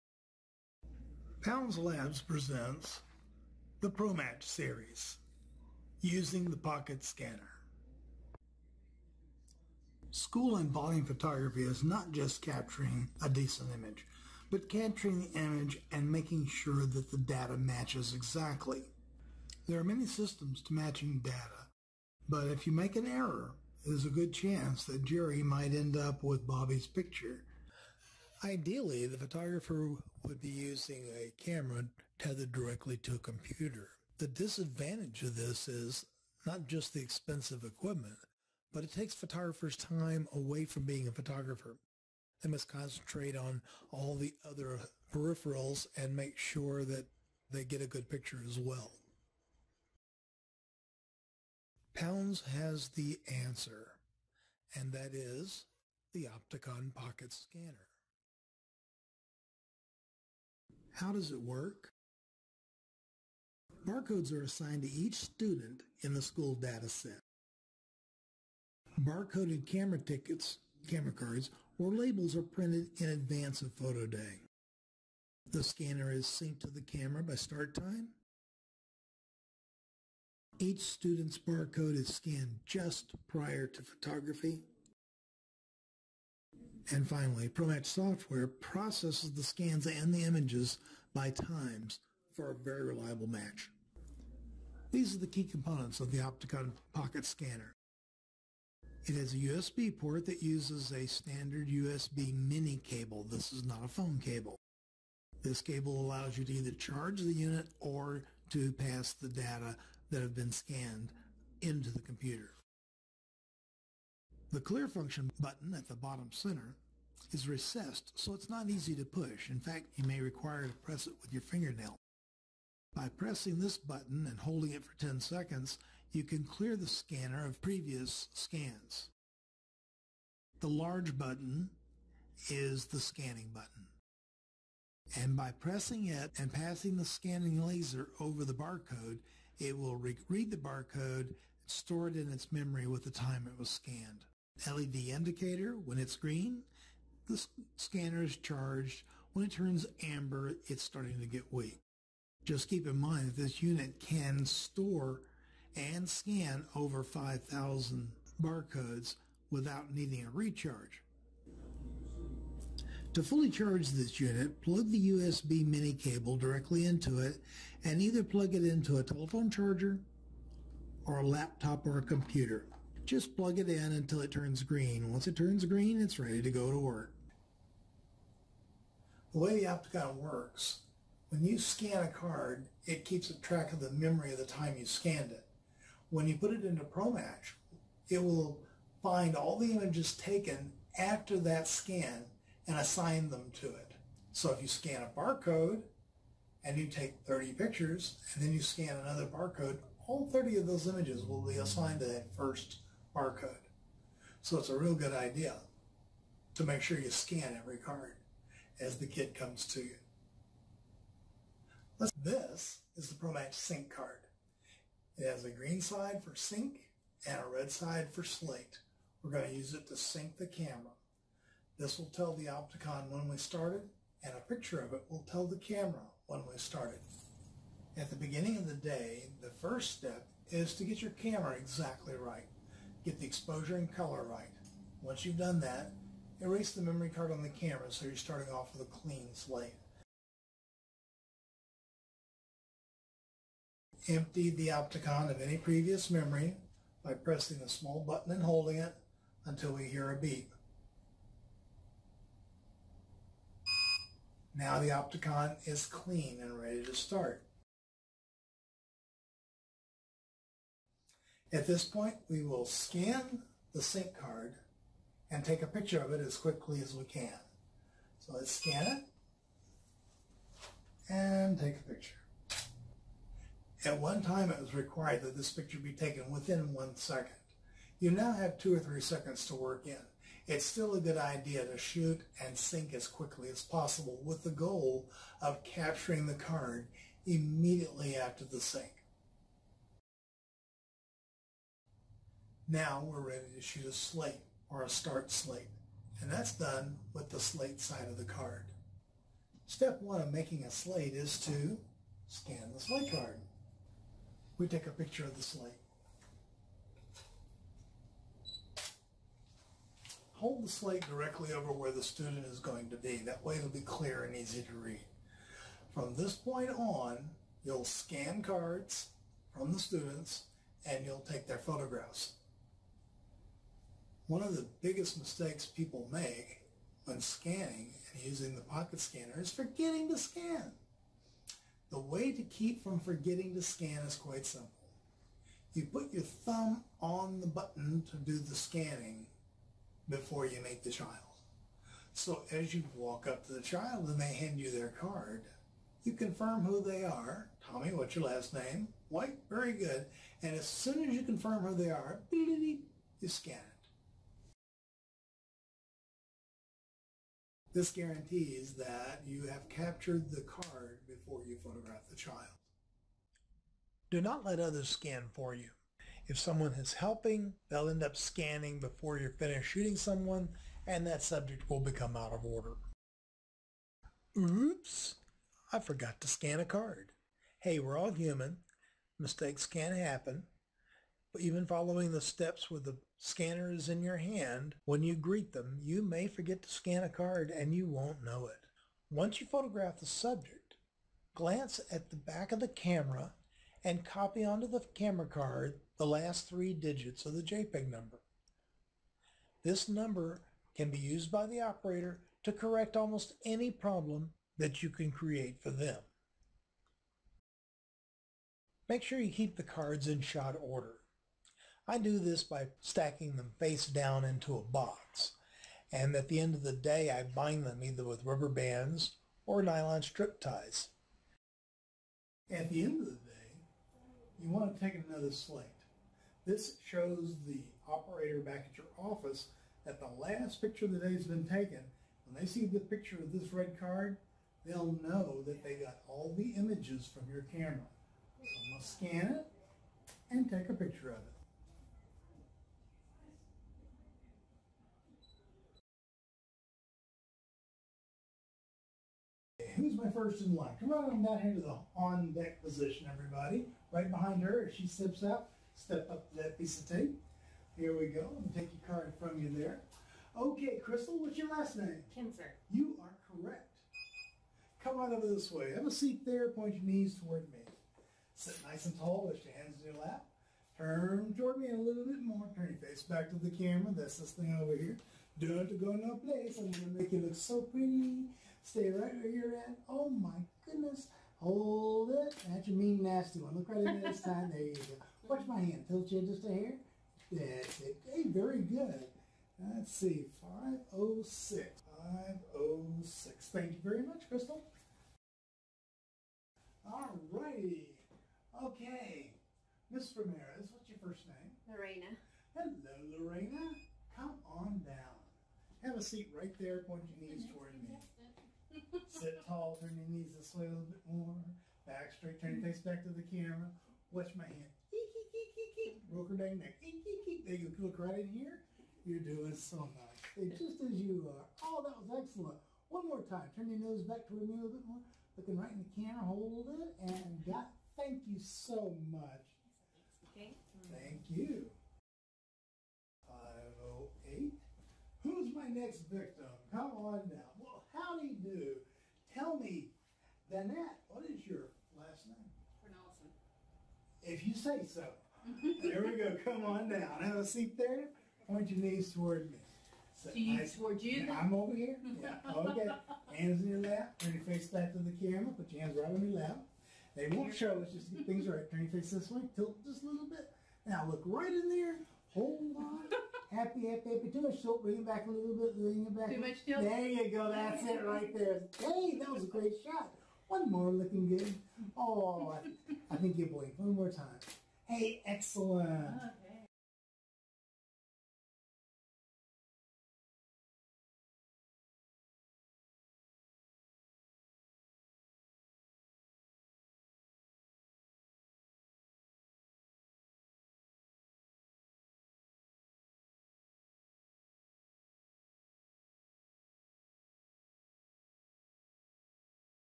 School and Sports Volume Photography - using an Opticon Scanner Matching Images and Data
Opticon Scanner for School and Sports Volume Photography.
Featuring Marion Hughes.
Description Coming Soon.
Demo
Scanning
Barcode
Slate Card
Sync Card
Matching Data to Images